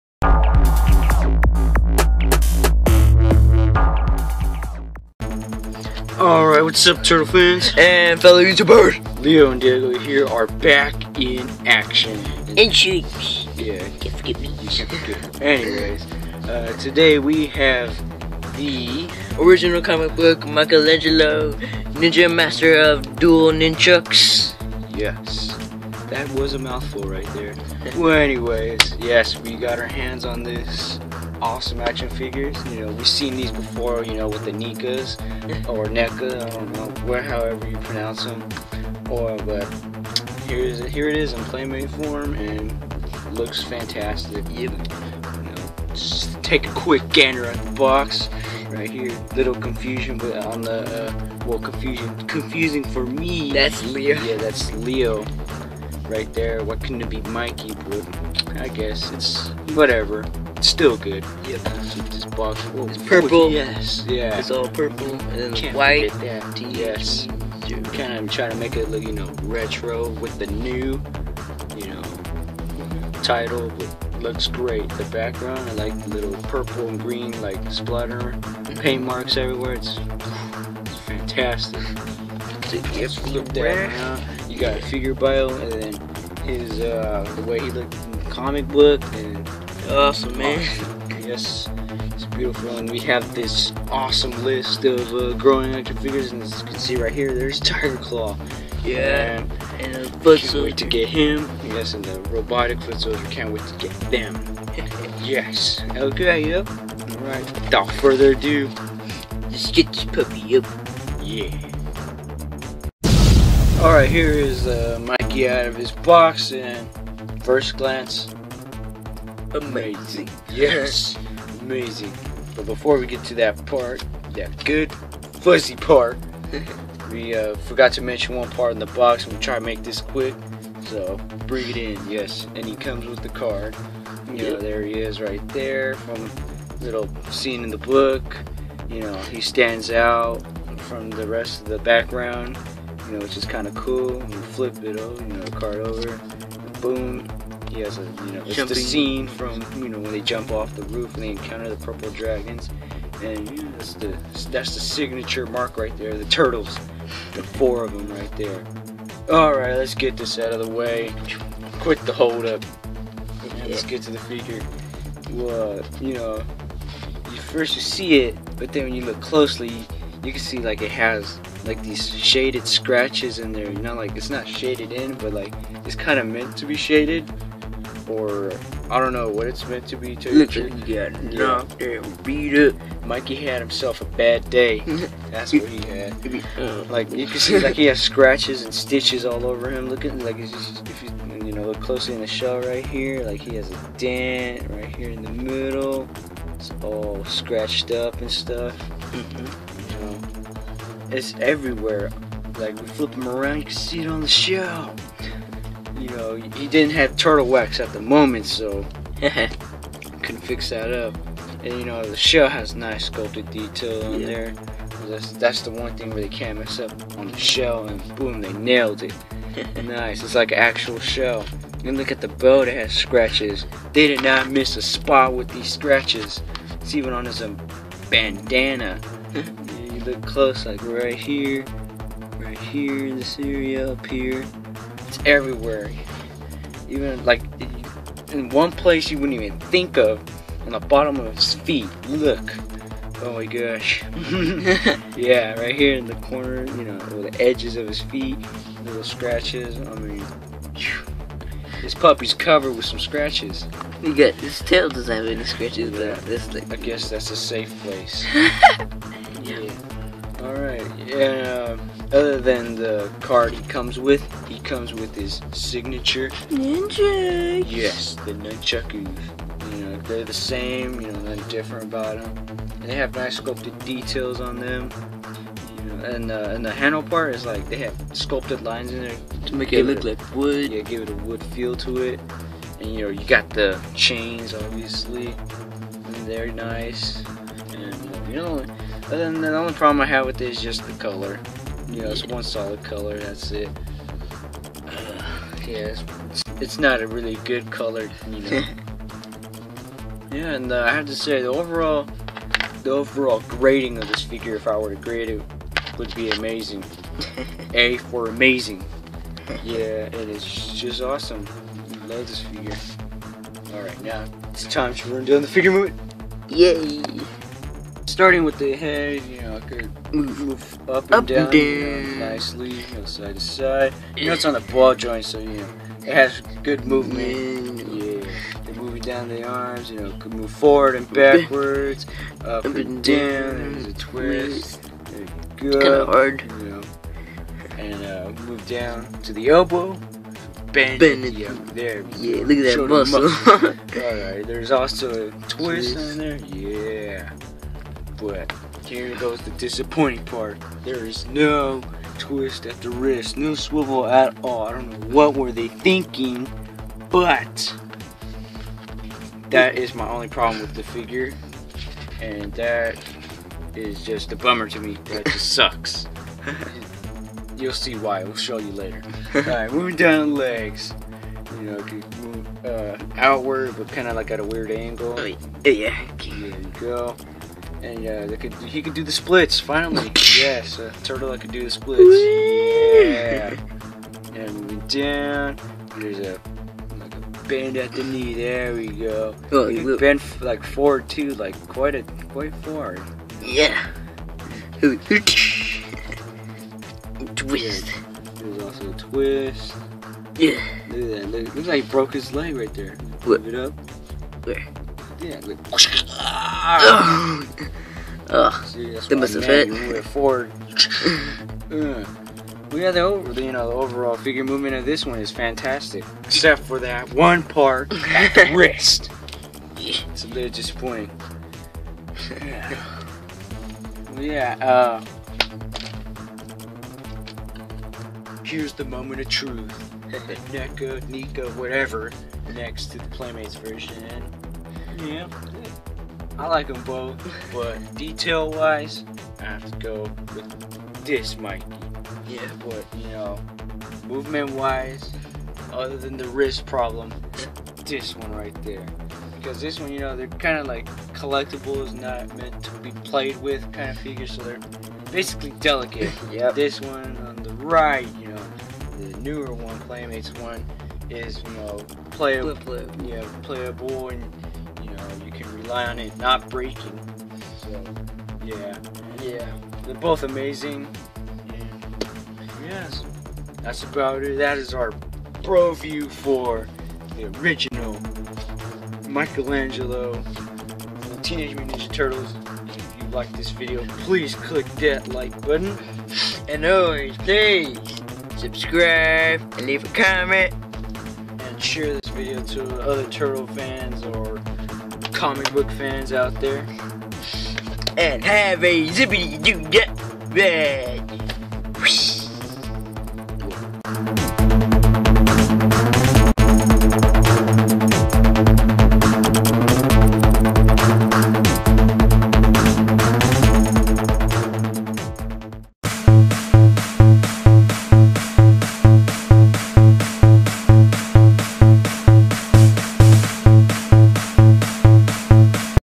Broview: Original Comic Book Michelangelo Nickelodeon TMNT 2014
Lugo bros Leo and Diego examine and grade the new comic book Michelangelo.